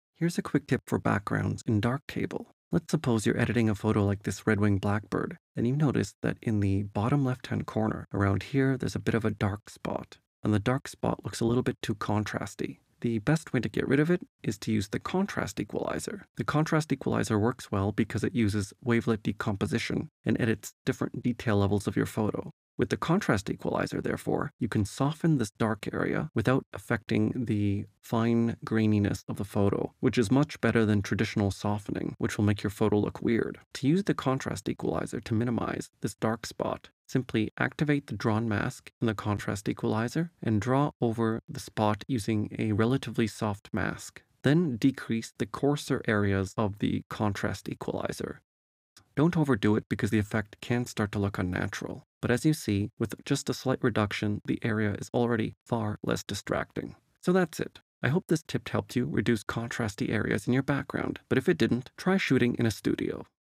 Improve Out of Focus Backgrounds With the Contrast EQ: Darktable
The contrast equalizer is one of the most useful tools in darktable, and is perfect for reducing contrast in the background to make backgrounds look a little better. Watch this quick tutorial to see how it's done!

And don't forget, the new and upgraded Udemy darktable course is coming soon!